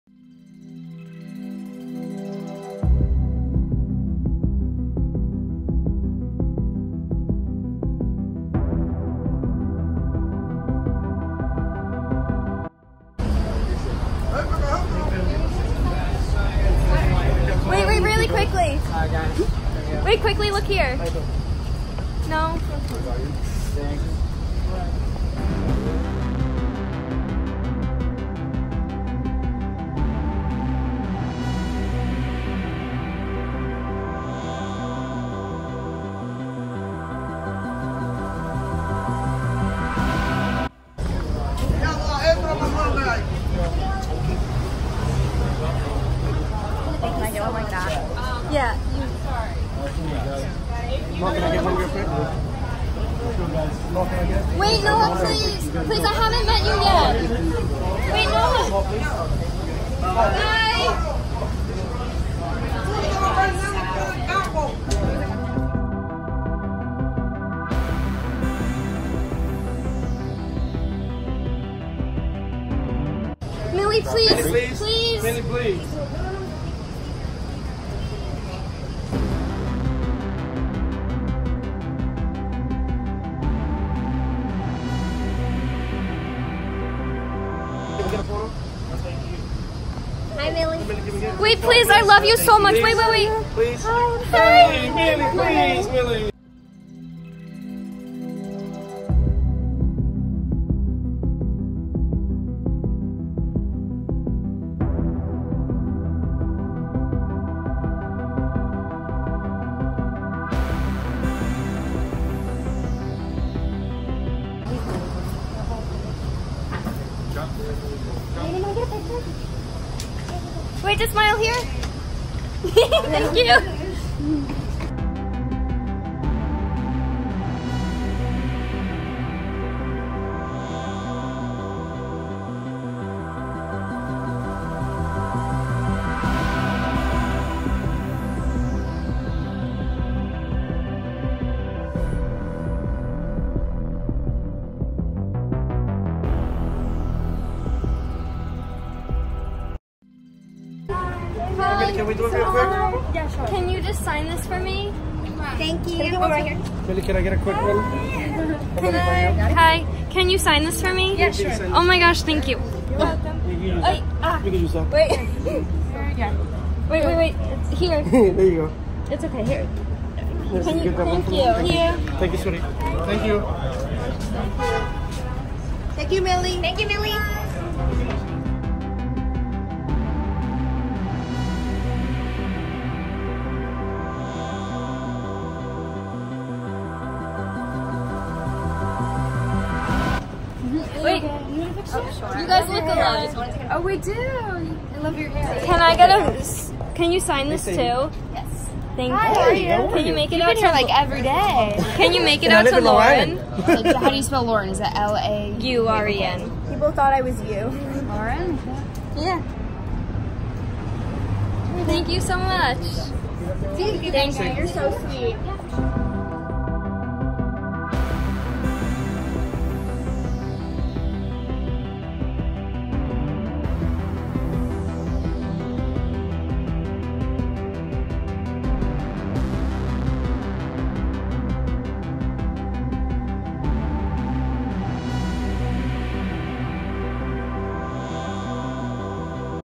MEETING THE STRANGER THINGS CAST!!!
I got to meet the Stranger Things cast in NYC!